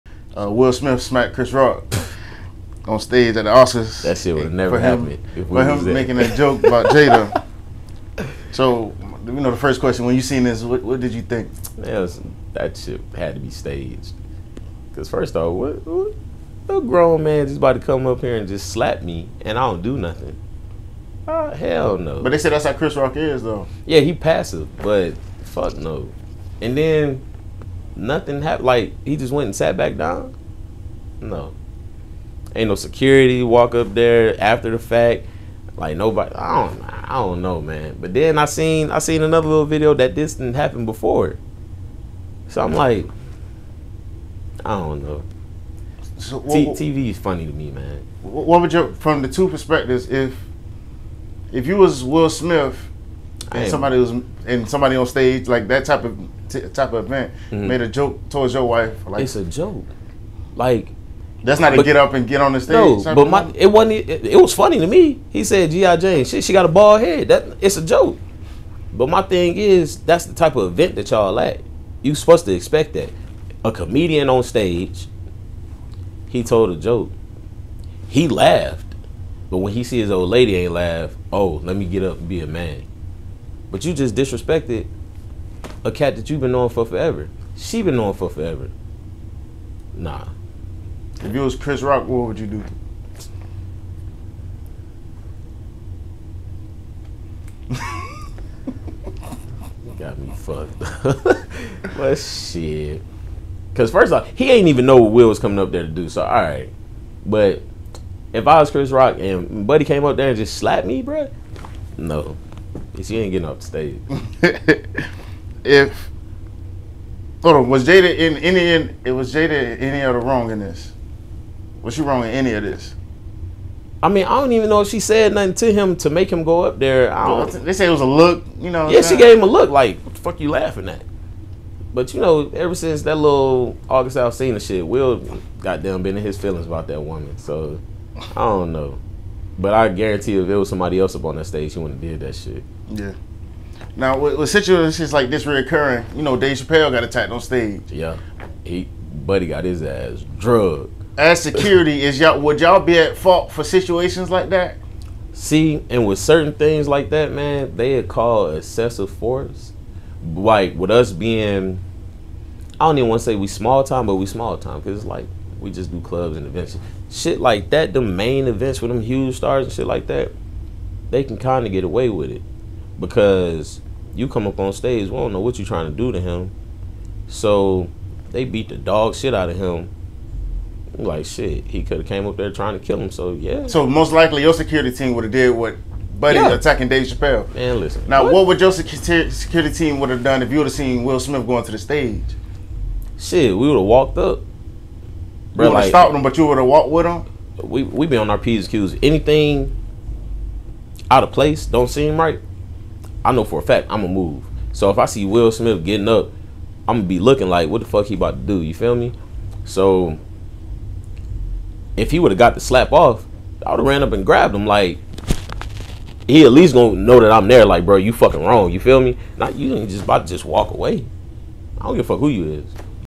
Devon (BBN Security) Talks Will Smith smacking Chris Rock + Talents being attacked on stage (CLIP)
MEMORIIIES MEDIA PODCAST

or
Memoriiies.com